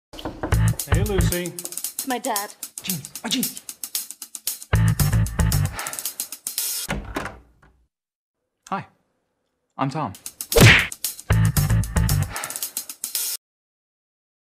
Wrigley's Extra Time To Shine - Parents PARODY
How Tom would be greeted by a real dad.